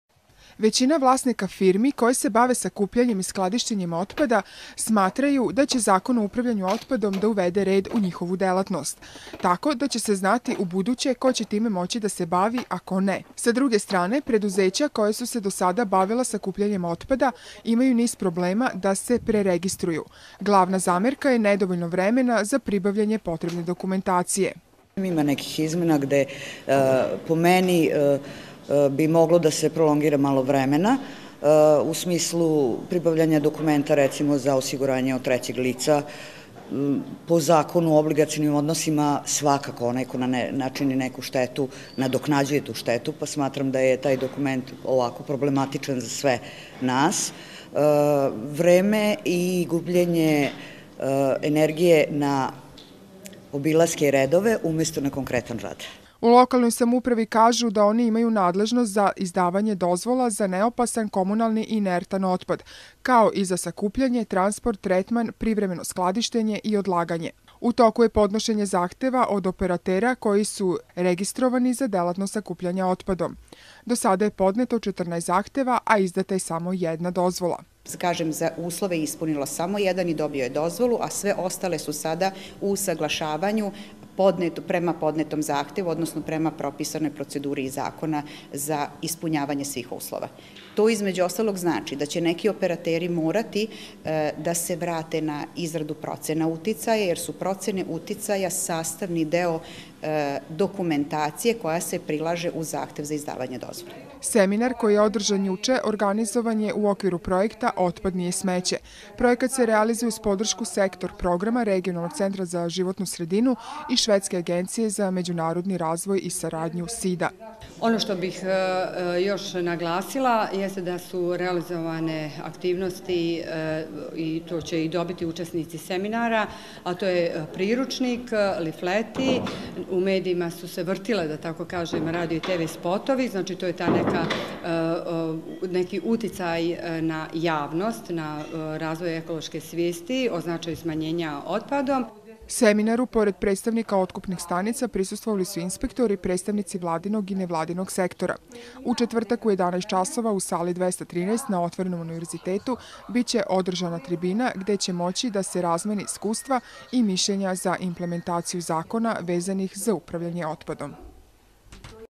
Prva dozvola za sakupljanje otpada
Preduzeće na Paliću dobilo je među prvima u zemlji dozvolu za sakupljanje, tretman, skladištenje otpada, a od Ministarstva za životnu sredinu tražiće i dozvolu za izvoz.Novi Zakon o upravljanju otpadom, koji je usvojen maja prošle godine dao je veća ovlašćenja lokalnim samoupravama koje izdaju dozvole otkupnim stanicama.Rok je bio da do kraja prošle godine sve otkupne stanice podnesu zahteve koji su registovani za delatnost sakupljanja otpada i uz njega prilože potrebnu dokumentaciju propisanu zakonom.Većina vlasnika firmi koje se bave sakupljanjem i skladištenjem otpada smatraju da će zakon o upravljanju otpadom da uvede red u njihovu delatnost.Tako da će se znati ubuduće ko će time moći da se bavi, a ko ne. Sa druge strane, preduzeća koja su se do sada bavila sakupljanje otpada imaju niz problema da se preregistruju.Glavna zamerka je nedovoljno vremena za pribavljanje potrebne dokumentacije.U Lokalnoj samoupravi kažu da oni imaju nadležnost za izdavanje dozvola za neopasan, komunalni i inertan otpad, kao i za sakupljanje, transport,tretman,privremeno skladišnje i odlaganje.U toku je podnošenje zahteva od operatera koji su registrovani za delatnost sakupljanja otpadom.Do sada je podneto 14 zahteva,a izdata je samo jedna dozvola.Seminar koji je održan juče organizovan je u okviru projekta Otpad nije smeće.Projekat se realizuje uz podršku Sektor programa Regionalnog centra za životnu sredinu i Švedske agencije za međunarodni razvoj i saradnju.Seminaru pored predstavnika otkupnih stanica prisustvovali su i inspektori, predstavnici vladinog i nevladinog sektora.U četvrtak u 11 časova u sali 213 na Otvorenom univerzitetu biće održana tribina gde će moći da se razmene iskustva i mišljenja za implementaciju zakona vezanih za upravljanje otpadom.